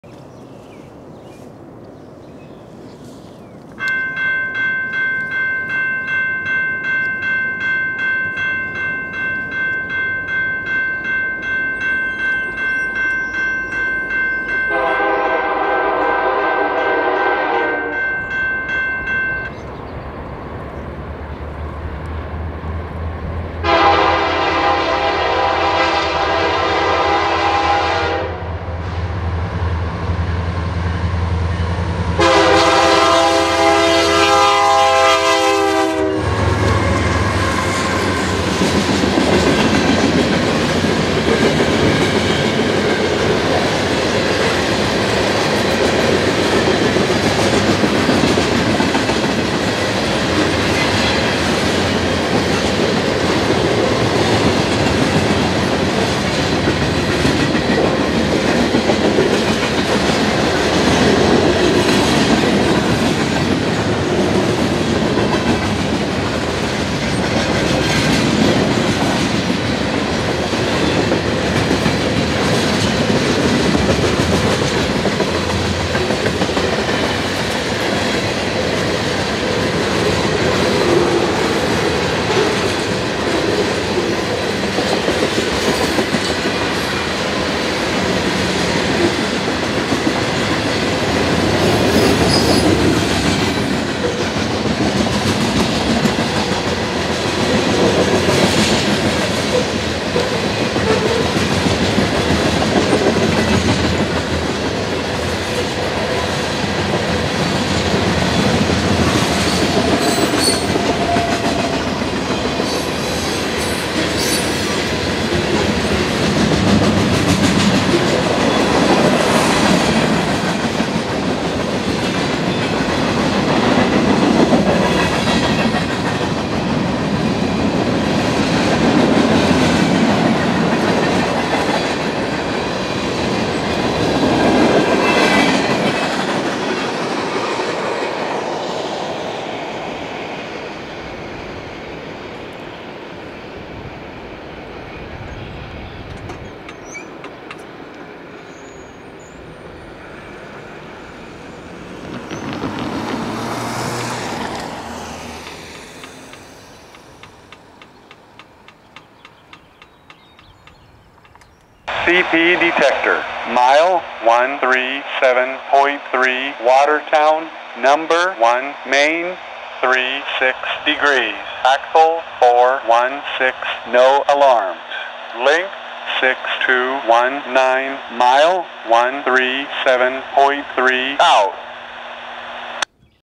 Canadian Pacific's Arid Region Tribute Unit leads Train 288 at Walton Road (with Defect Detector)
CP 7021 and CP 8940 lead train 288 east, down Canadian Pacific's Watertown Subdivision, between Reeseville and Richwood, WI.  The milepost 137.3 defect detector gives a readout after the train passes.  This was filmed at the Walton Road railroad crossing.